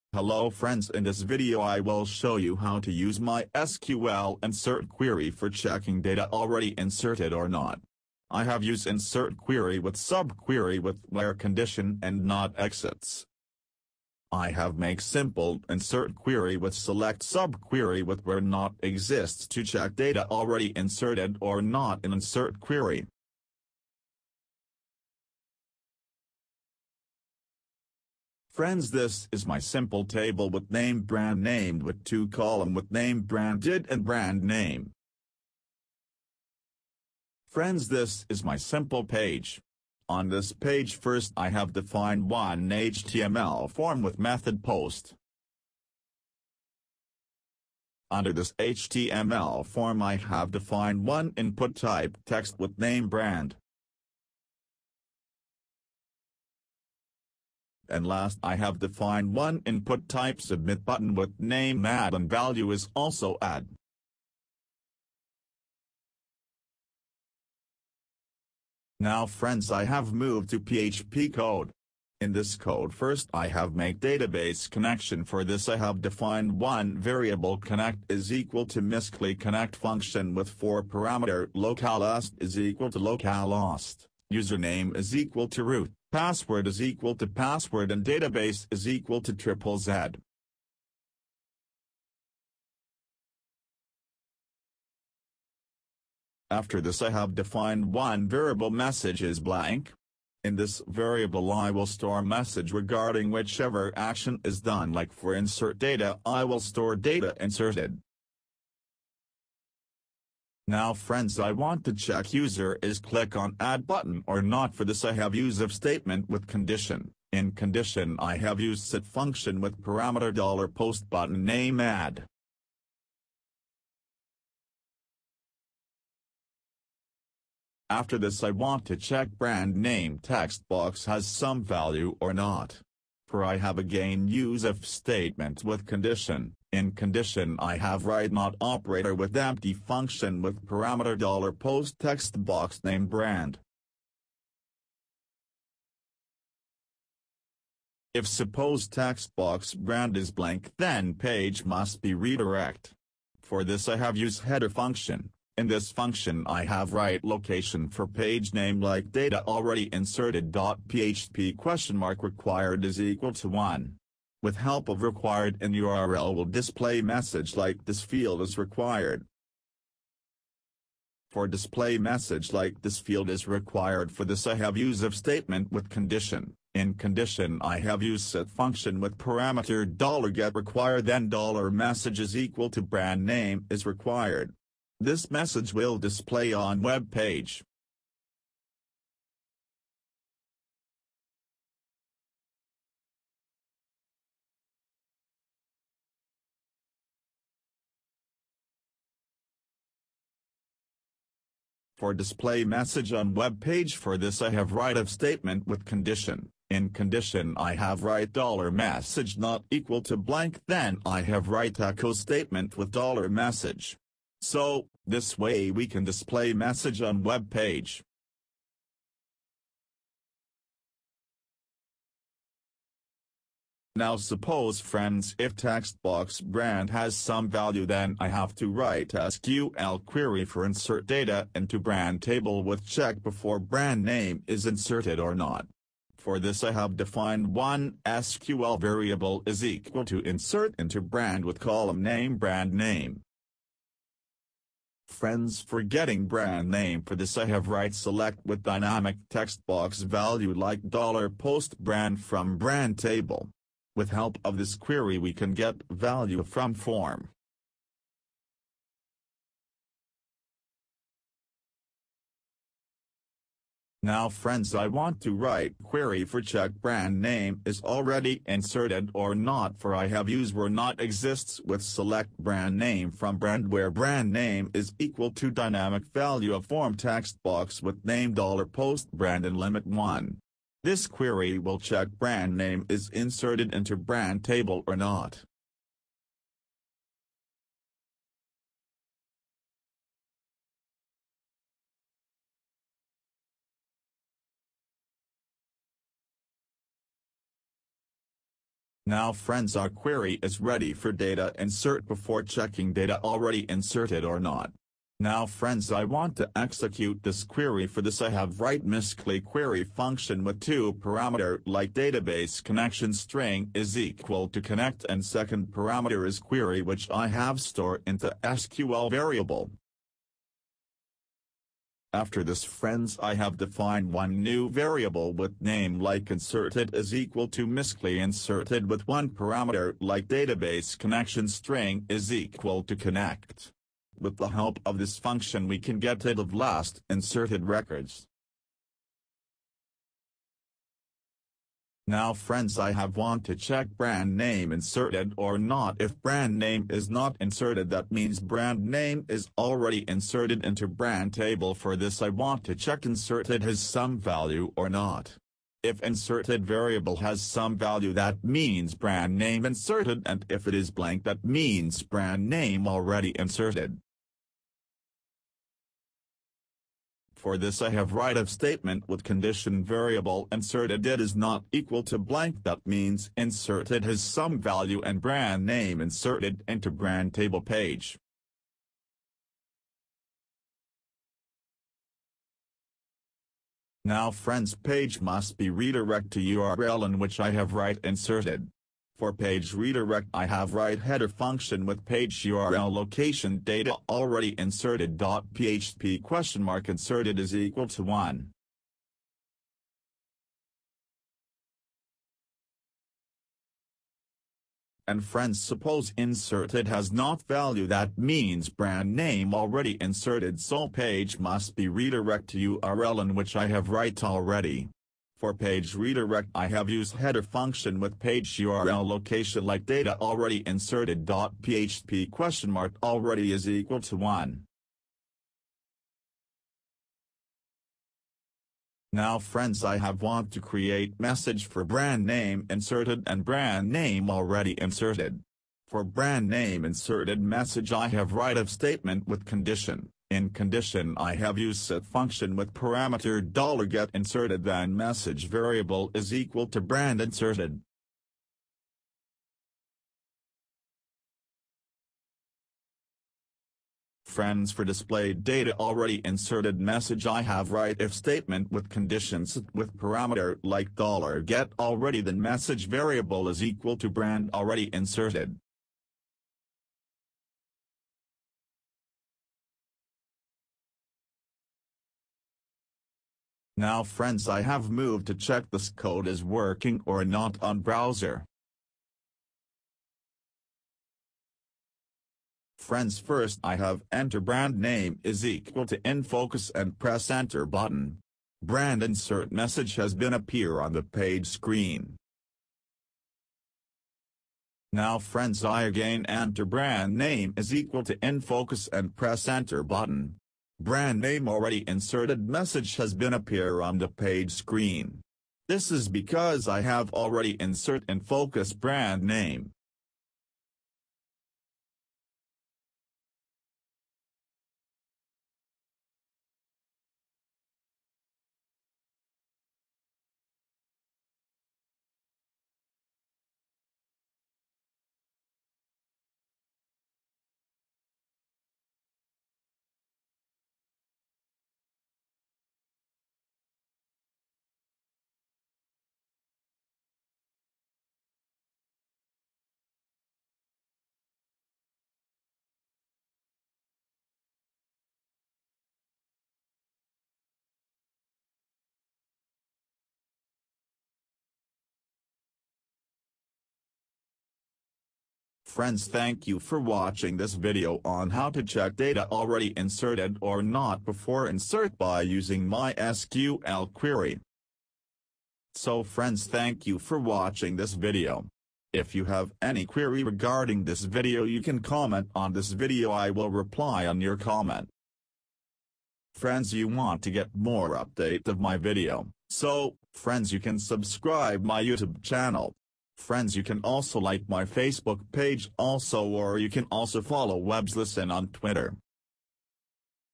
PHP MySQL Insert record if not exists in table
In this video you can find how to use php mysql insert query for checking data already inserted or not. I have use insert query with sub query with where condition and not exits. I have simple use inner join in this insert query for checking data already inserted or not. Source Code